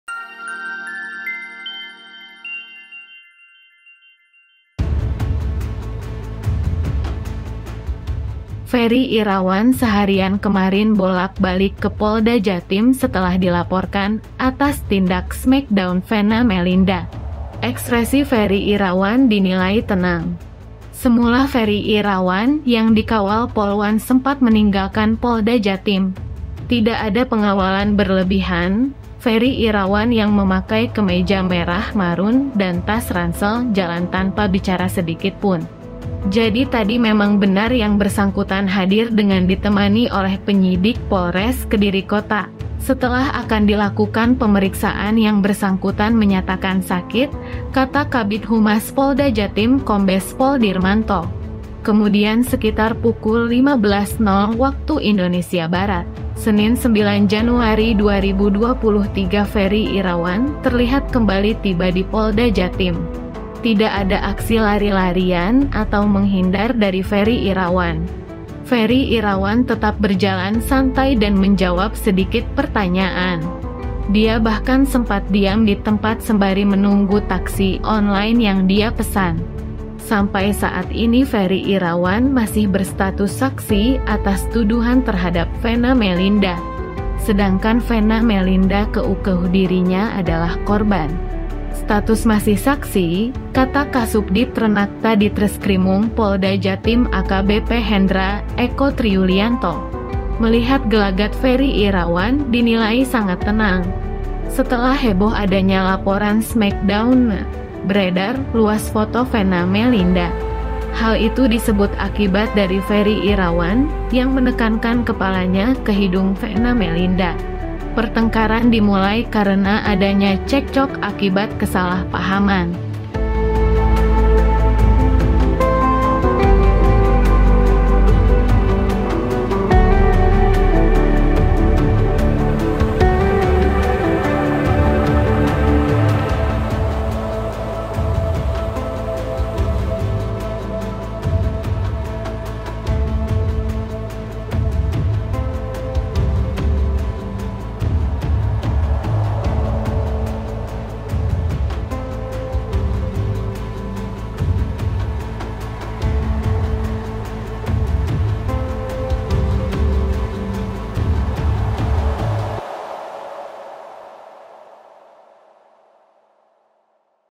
VIRAL ! Ekspresi Ferry Irawan Menjadi Sorotan Saat Diperiksa Polisi
Ekspresi Ferry Irawan Menjadi Sorotan Saat Diperiksa Polisi

Ferry Irawan seharian kemarin bolak-balik ke Polda Jatim setelah dilaporkan atas tindak smack down Venna Melinda. Ekspresi Ferry Irawan dinilai tenang.

Semula Ferry Irawan yang dikawal Polwan sempat meninggalkan Polda Jatim. Tidak ada pengawalan berlebihan, Ferry Irawan yang memakai kemeja merah marun dan tas ransel jalan tanpa bicara sedikitpun.

"Jadi tadi memang benar yang bersangkutan hadir dengan ditemani oleh penyidik Polres Kediri Kota. Setelah akan dilakukan pemeriksaan yang bersangkutan menyatakan sakit," kata Kabid Humas Polda Jatim Kombes Pol Dirmanto.

Kemudian sekitar pukul 15.00 WIB, Senin (9/1/2023) Ferry Irawan terlihat kembali tiba di Polda Jatim. Tidak ada aksi lari-larian atau menghindar dari Ferry Irawan.

Ferry Irawan tetap berjalan santai dan menjawab sedikit pertanyaan. Dia bahkan sempat diam di tempat sembari menunggu taksi online yang dia pesan.

Sampai saat ini Ferry Irawan masih berstatus saksi atas tuduhan terhadap Venna Melinda.